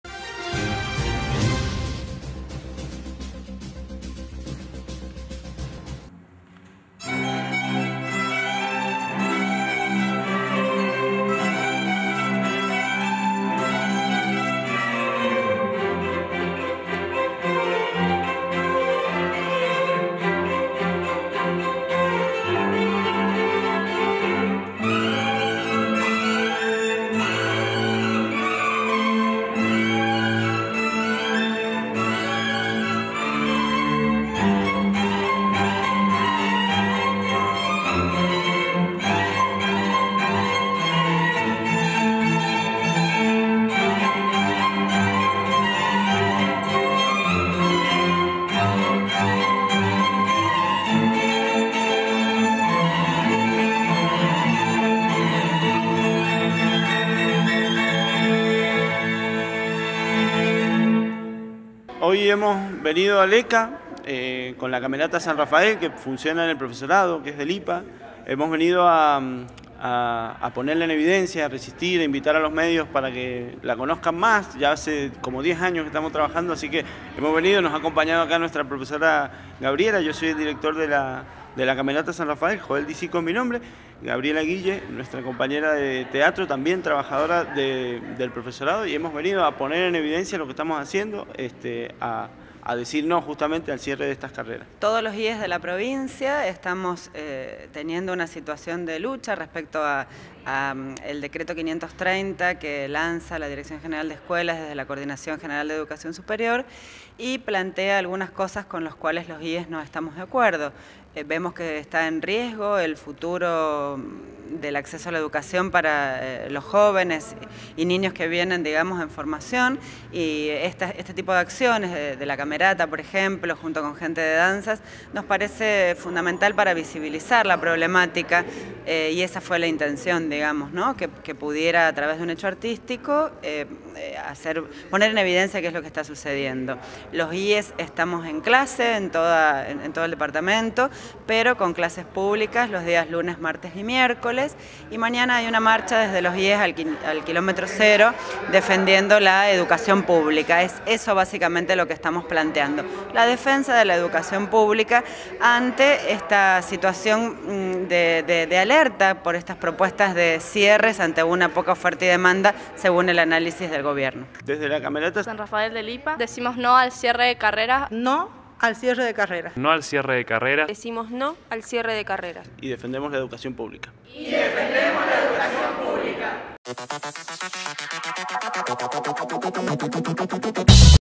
CAMERATA COMPROMETIDA
La orquesta del IPA se comprometió públicamente con la lucha docente y estudiantil de Terciarios y Universidades brindando desde el ECA Sur su opción ante el generalizado problema existente.